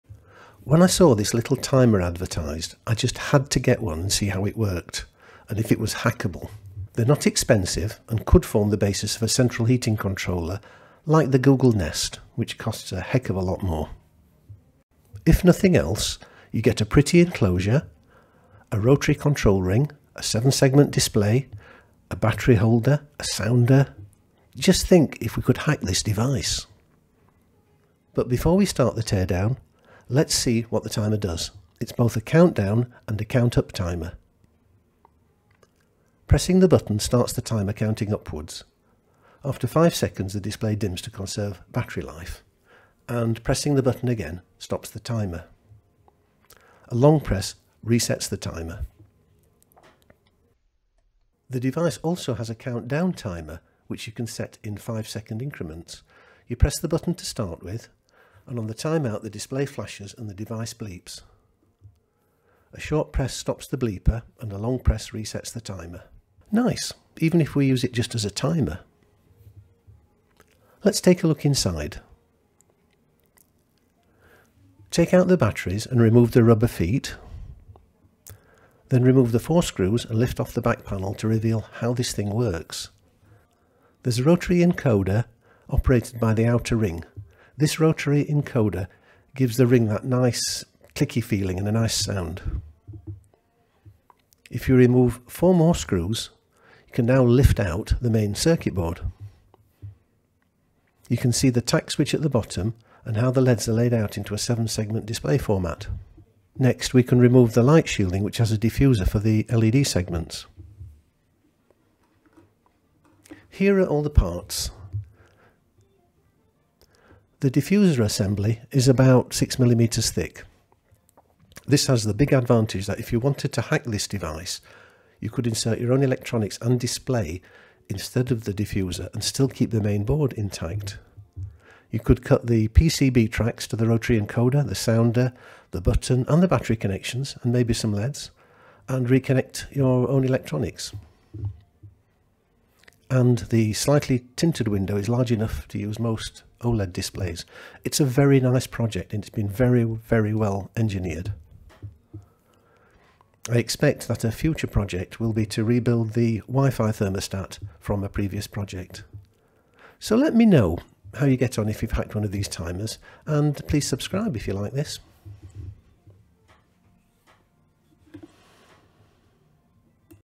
Can this cheap timer hardware be converted into a smart rotary control with built-in display?
These cheap rotary timers have loads of goodies built-in: rotary encoder, push switch, battery compartment, piezo sounder, LCD or OLED display, and plenty of room for your own circuit board.

This is an incredably cheap way of getting a quality enclosure.

The uses are only limited by your imagination - hack and use for anything that can be controlled by a rotary encoder and a push-switch. You can even build in your own LCD or OLED display.

This rotary-dial timer looks ideal for a wi-fi controller heating thermostat. It should have enough space inside for your most of you project electronics.

Uses for a rotary control and push switch?
LIMITLESS !